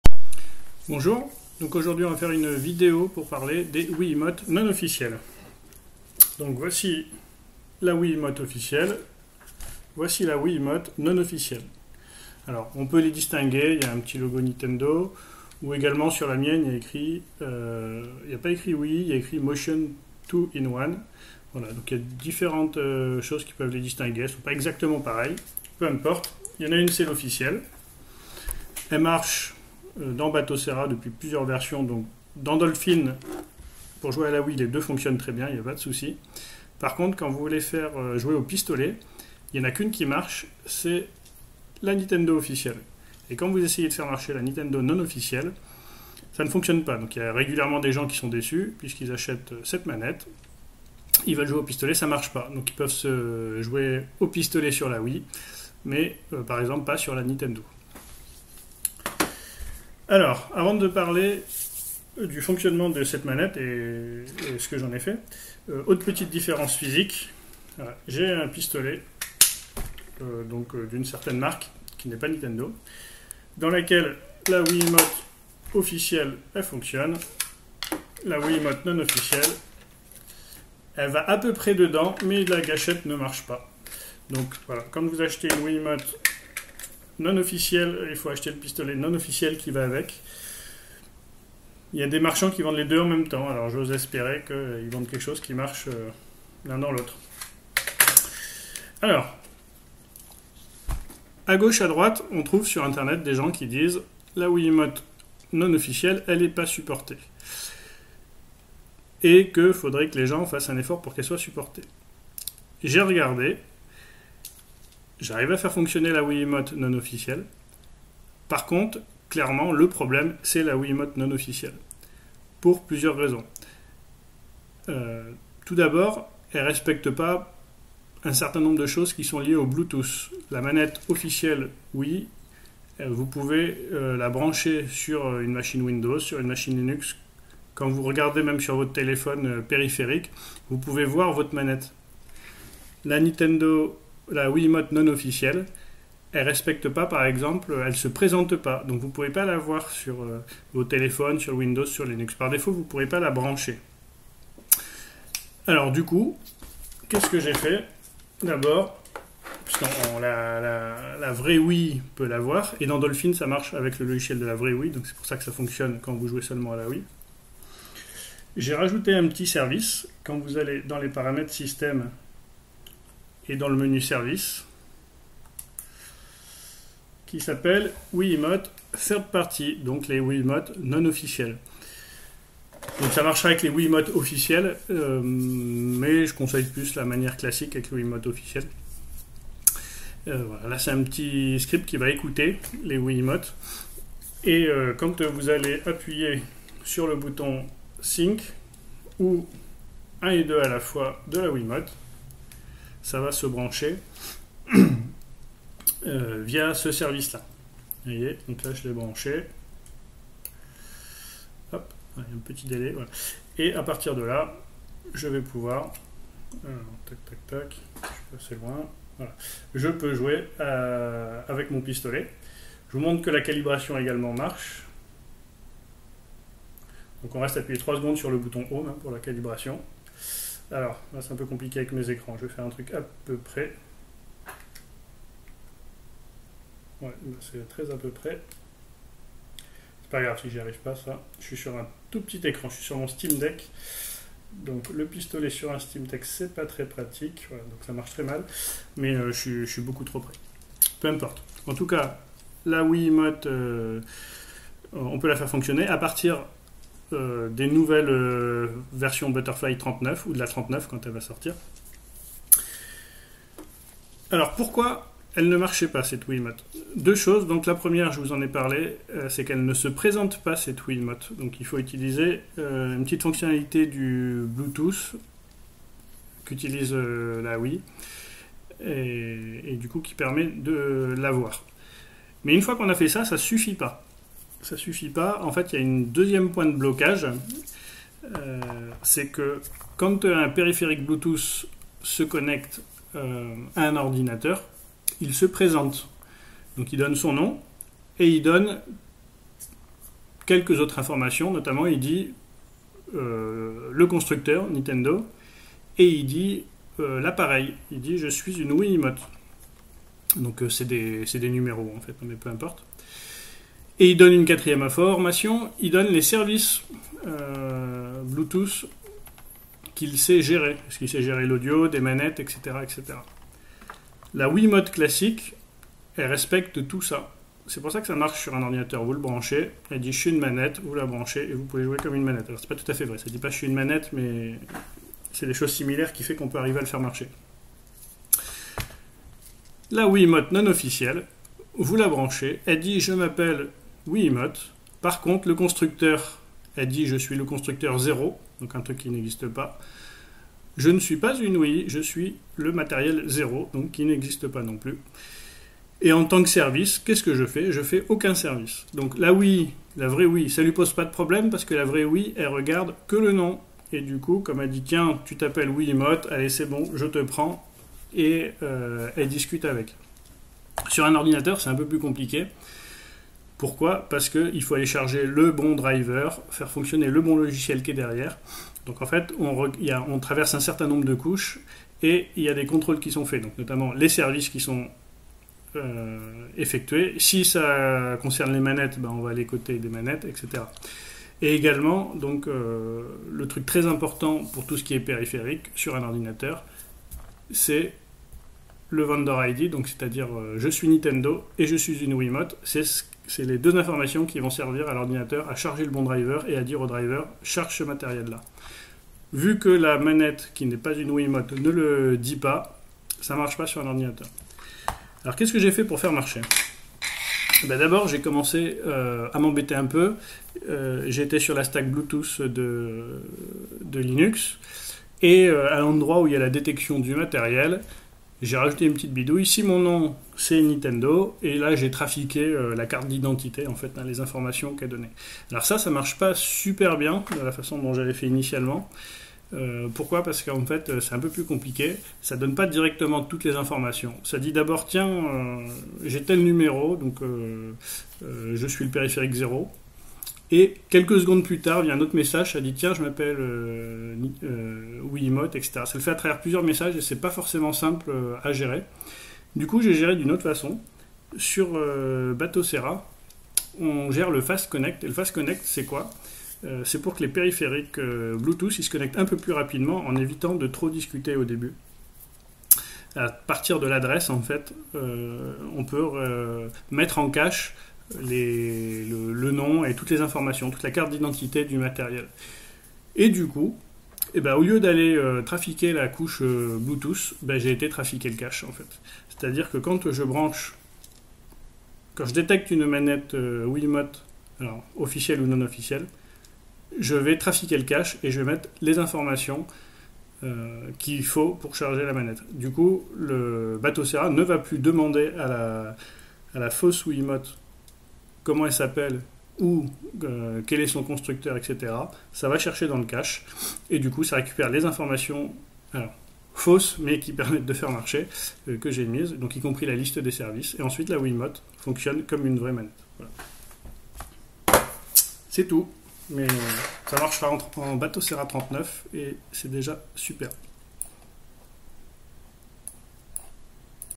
Les wiimotes non officielles en gun sur batocera
Dans cette vidéo, je vous montre mon travail de la semaine dernière pour faire fonctionner les manettes wii non officielles en gun sur batocera.
Le plus simple est de passer en version 39 butterfly pour l'utiliser, meme si techniquement, en copiant 2 fichiers, on peut le faire en 38.

A noter que certaines wiimote officielles marchent très bien également ; elles ne se valent pas toutes.